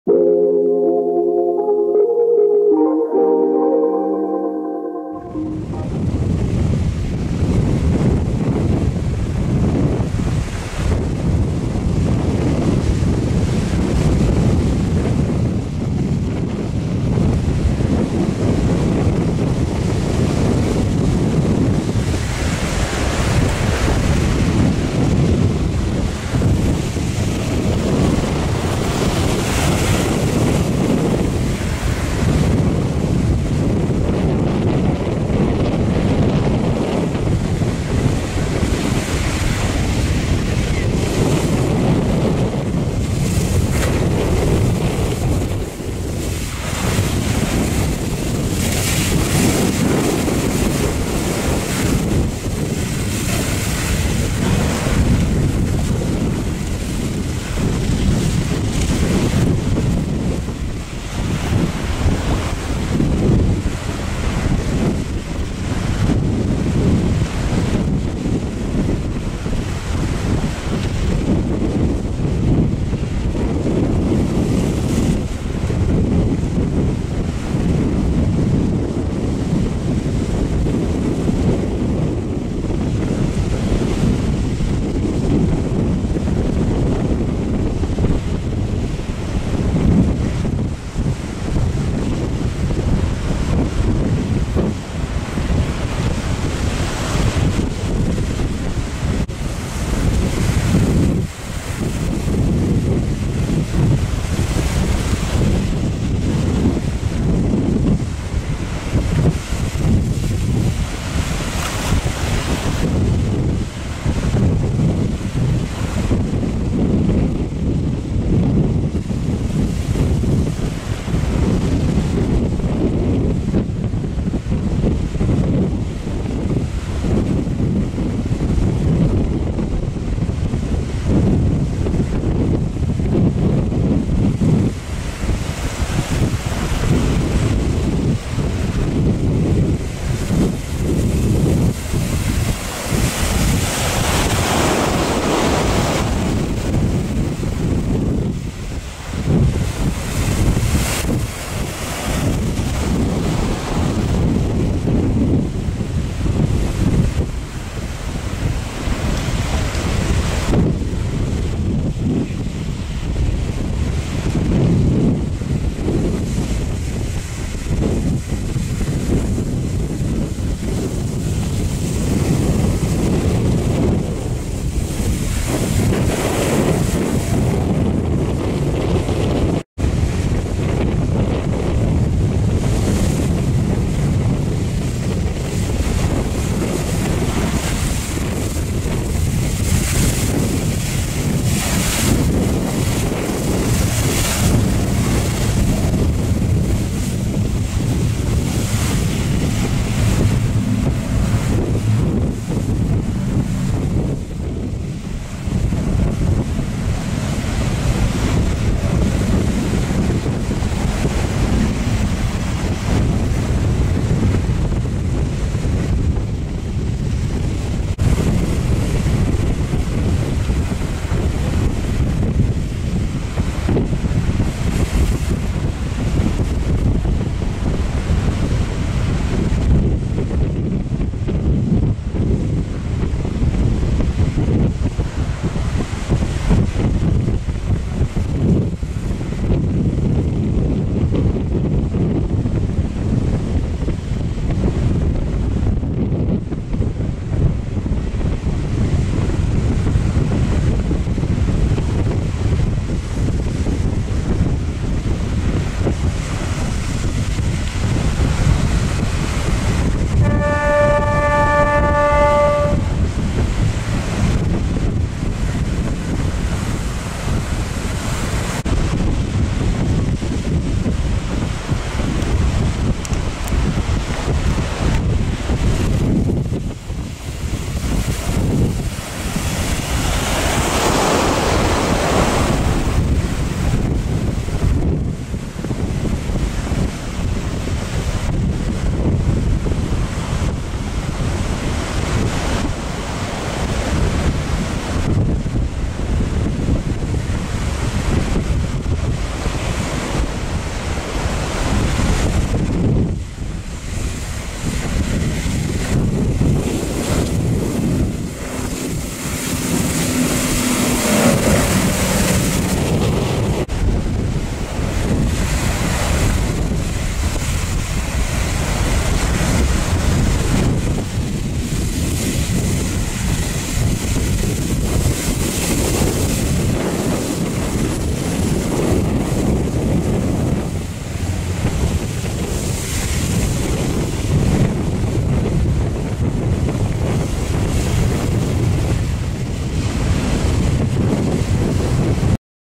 💯 OCÉAN TRÈS MOUVEMENTÉ CE MATIN À CAPBRETON ACCROCHEZ-VOUS. 🌊👀
💯 VERY ACTIVE OCEAN THIS MORNING IN CAPBRETON HANG ON. 🌊👀